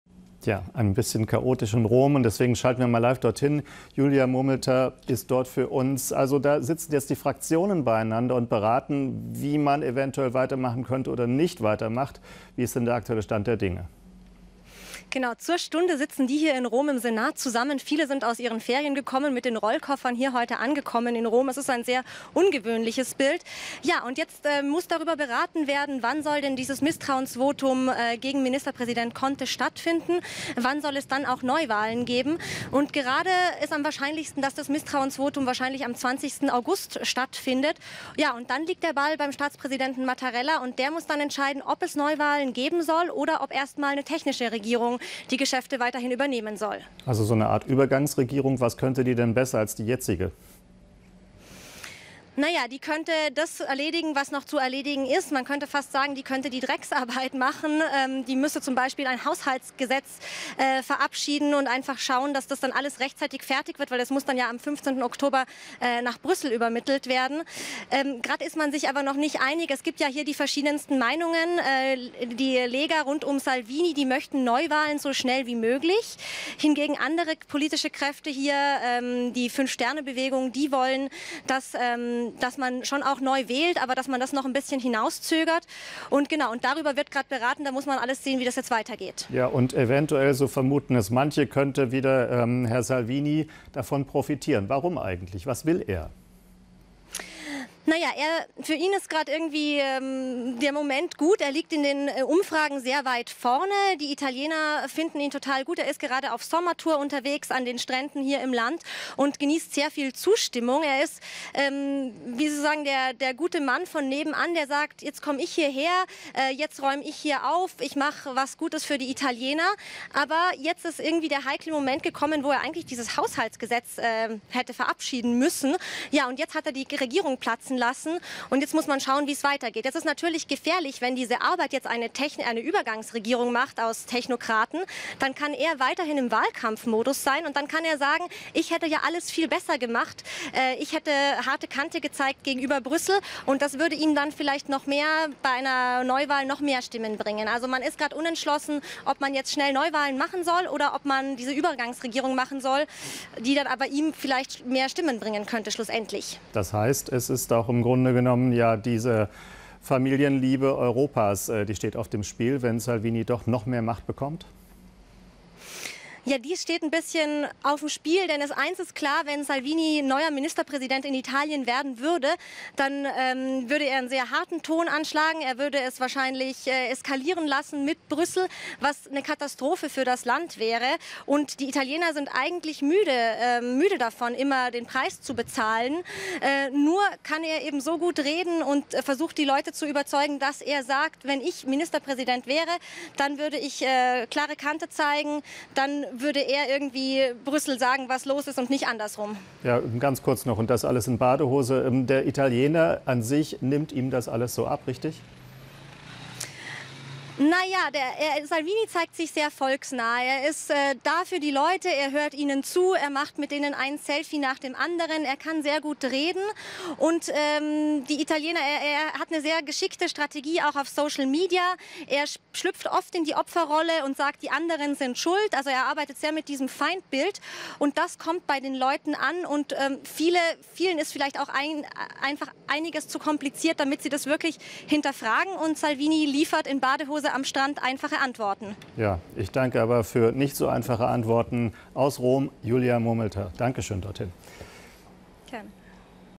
Italien: Julia Mumelter zur Rolle Matteo Salvinis in der aktuellen Regierungskrise am 12.08.19
Julia Mumelter (Journalistin ARD/BR München) im Schaltgespräch mit Moderator Stephan Kulle zum Misstrauensvotum gegen den italienischen Ministerpräsidenten Giuseppe Conte und der Rolle des Lega-Chefs Matteo Salvini.